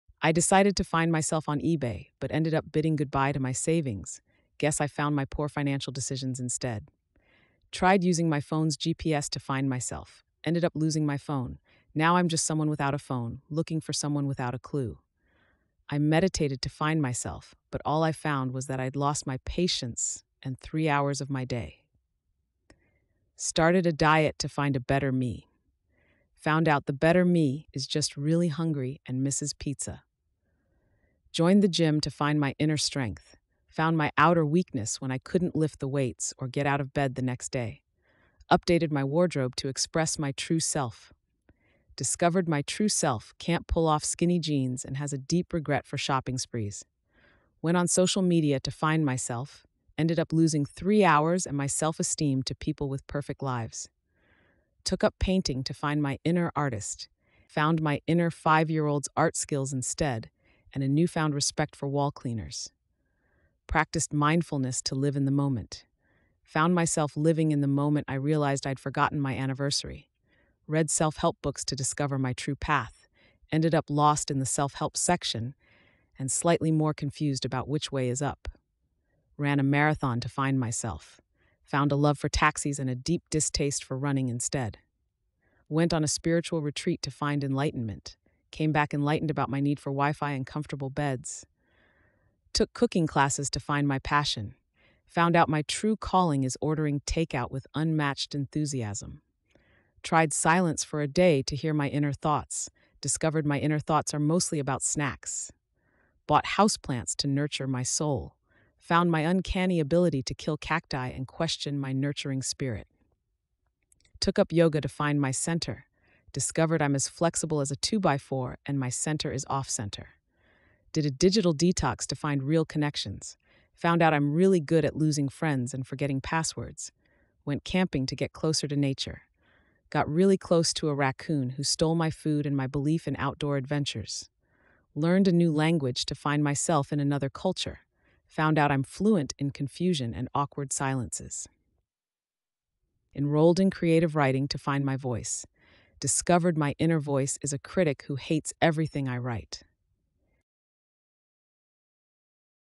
Quest for Self: The Comically Misguided Adventures of Personal Improvement
Embark on a hilarious journey with 'Quest for Self: The Comically Misguided Adventures of Personal Improvement,' a series that dives into the often amusing and always relatable world of self-discovery gone awry. From meditation mishaps and dieting disasters to the challenges of new hobbies and the pitfalls of technology, each episode brings to life the whimsical tales of our quest for betterment with a healthy dose of humor. Perfect for anyone who's ever tried and stumbled on the path to self-improvement, this series offers a light-hearted look at the missteps and miscalculations that make us human. Join us as we explore the laughter in our failures and the lessons learned in the least expected ways.